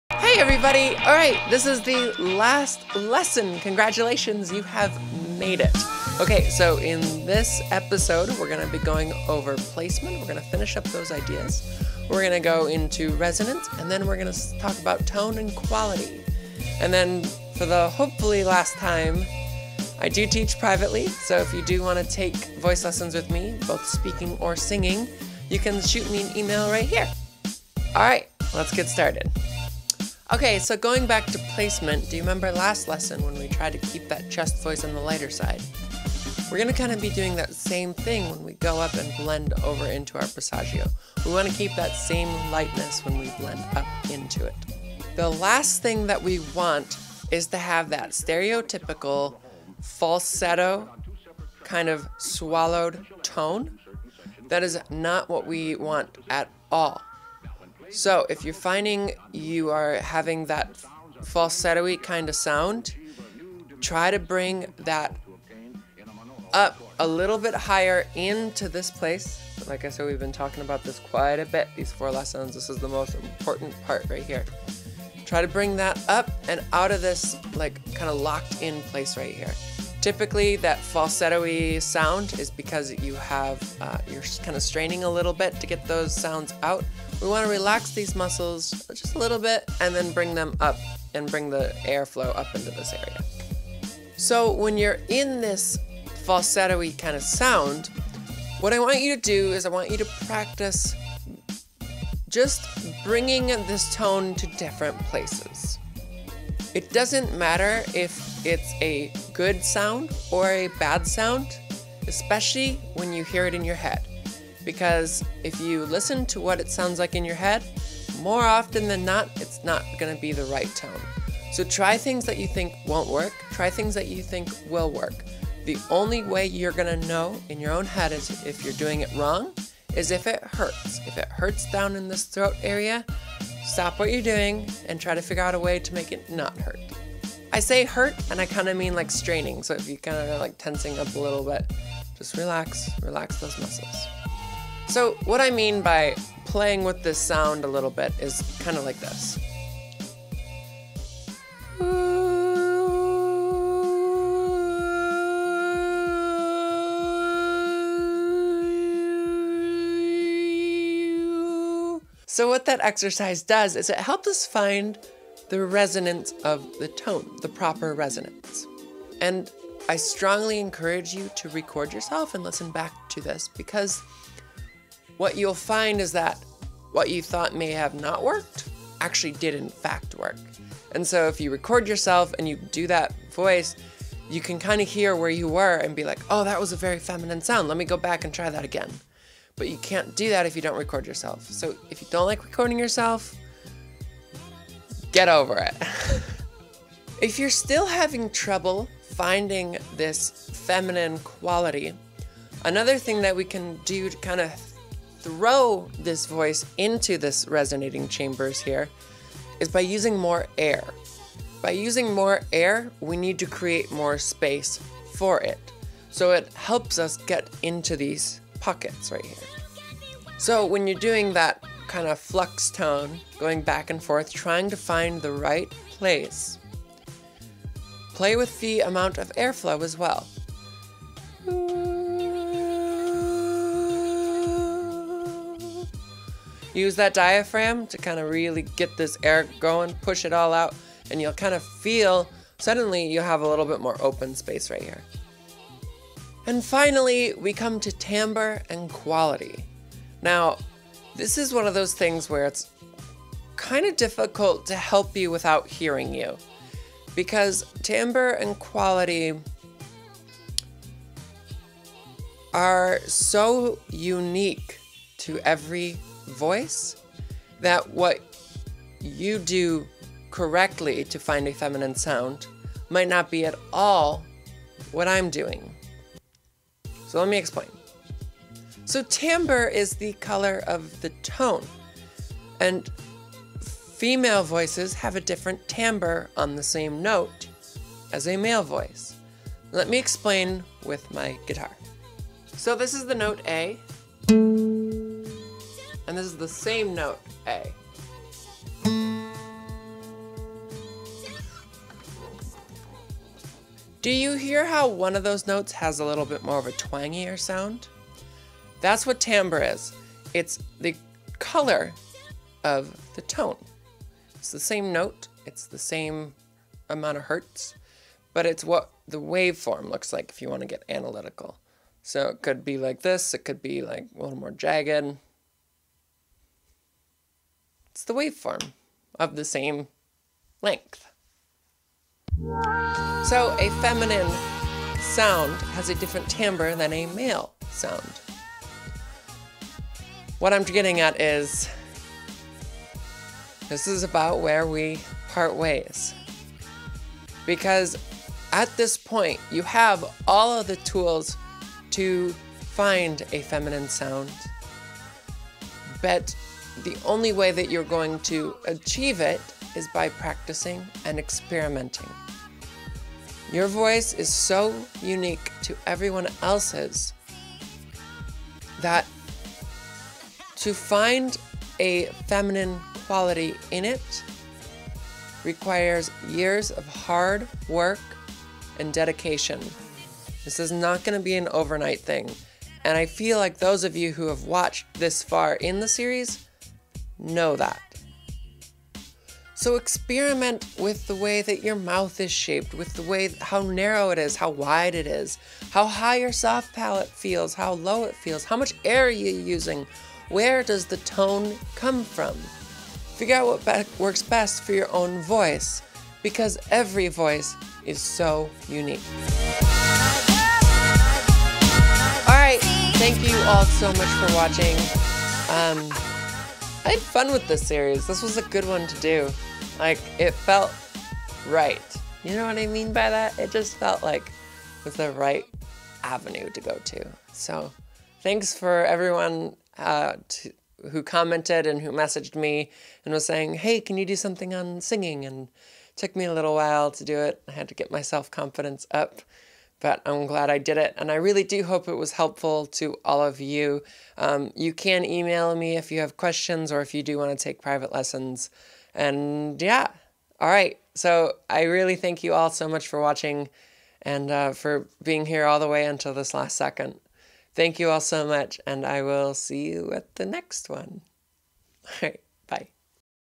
Trans Singing 401
The last lesson we talk a little bit more about placement and timbre.

Sign my Newsletter!